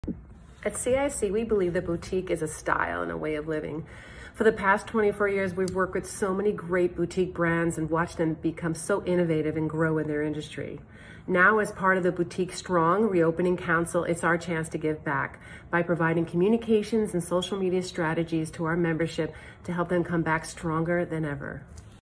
Carolyn Izzo-Feldman is BoutiqueStrong
Carolyn Izzo-Feldman, Founder of CIIC PR is a part of BLLA's BoutiqueStrong Council that is a force helping boutique hotels move forward in the new paradigm.